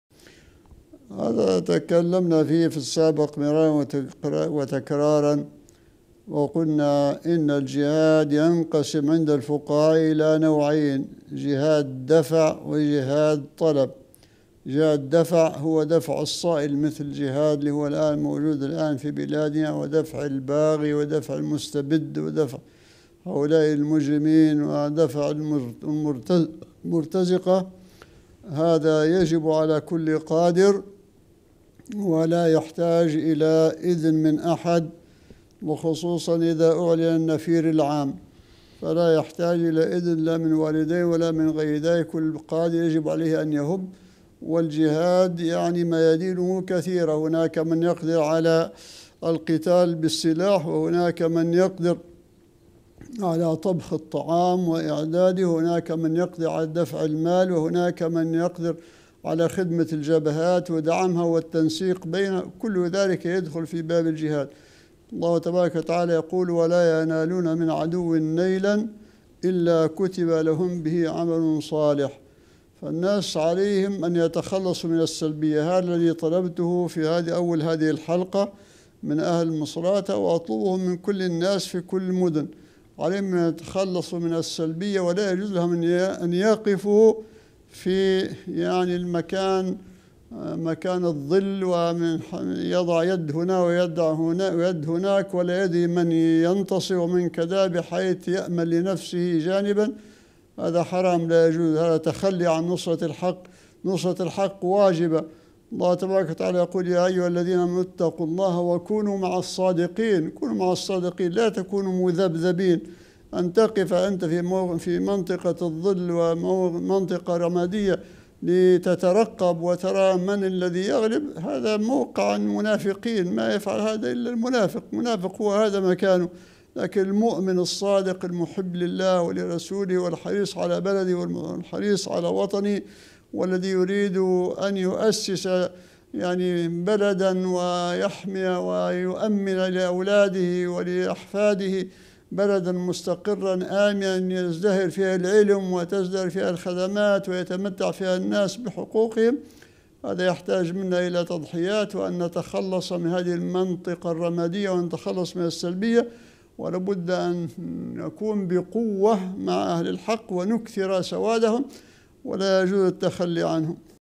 من الإسلام_والحياة | الشيخ د. الصادق_الغرياني: جهاد الدفع | 3 - 06 - 2020
ــــــــــــــــــــــــــــــــــــــــــــــــــــــــــــــــــــــــــ
ــــــــــــــــــــــــــــــــــــــــــــــــــــــــــــــــــــــــــ
ــــــــــــــــــــــــــــــــــــــــــــــــــــــــــــــــــــــــــ
يمكنكم تحميل تطبيقنا TANASUH TV على الهواتف الذكية: